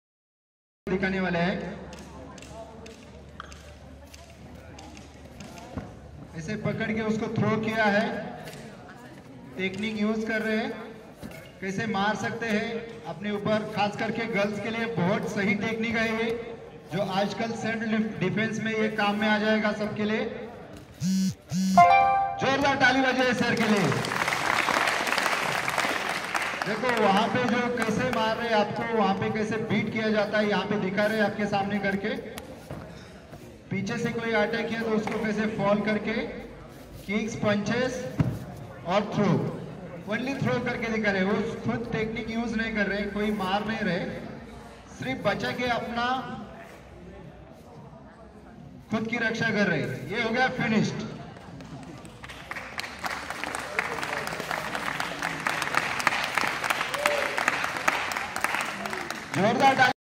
2018 Ashihara Karate Nage kata application in India
An impromptu demonstration of Ashihara Karate's Nage no Kata in application at the 2018 All India Ashihara Karate National Championships held in Pune in May 2018. Performed by Kaicho Hoosain Narker with Rajesh Jha of West Bengal as the attacker.